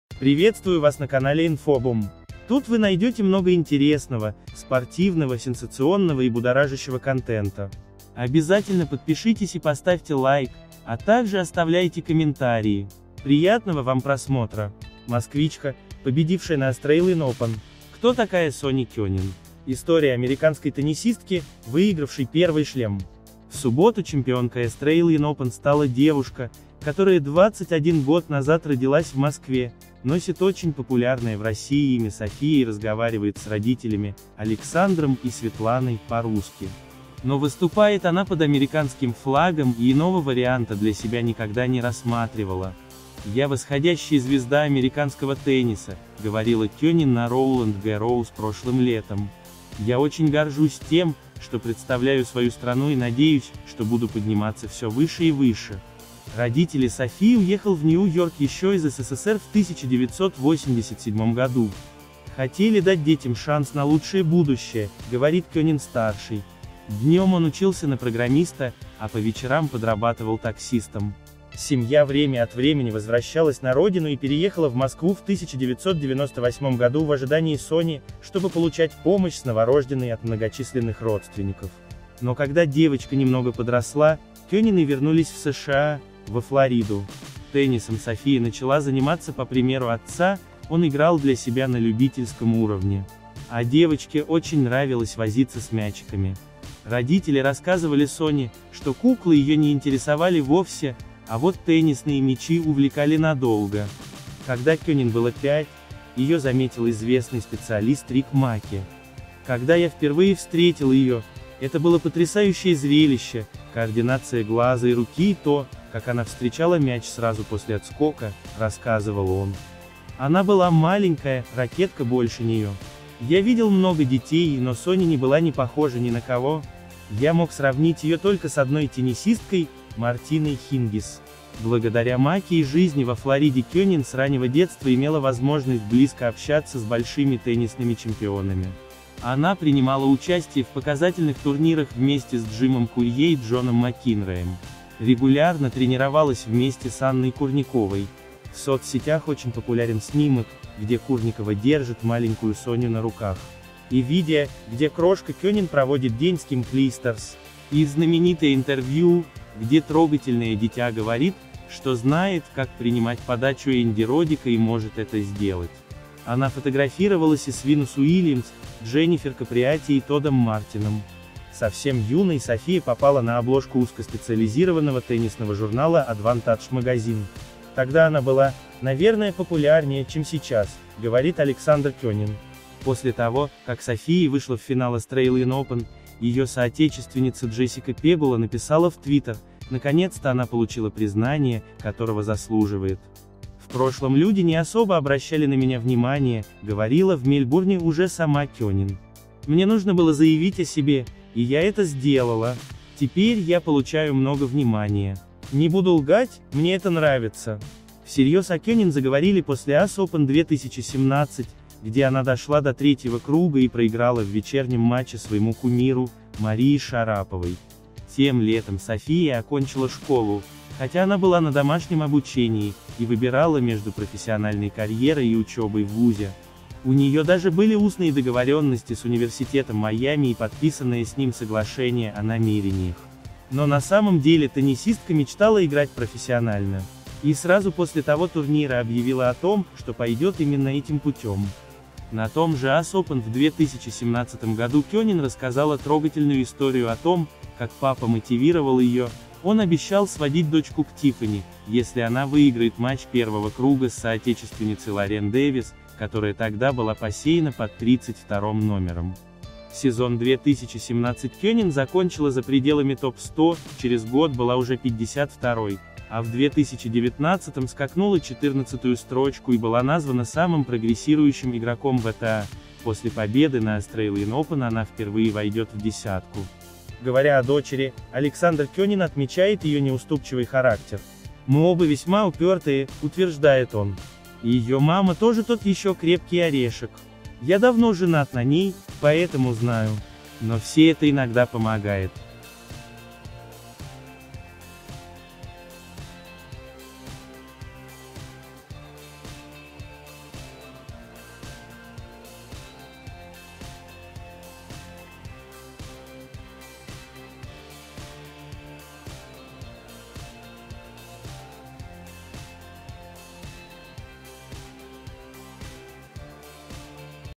Москвичка, победившая на Australian Open. Кто такая Соня Кенин?
В видео София Кенин которая в субботу стала чемпионкой Australian Open  девушка, которая 21 год назад родилась в Москве, носит очень популярное в России имя София и разговаривает с родителями — Александром и Светланой — по-русски. Но выступает она под американским флагом и иного варианта для себя никогда не рассматривала.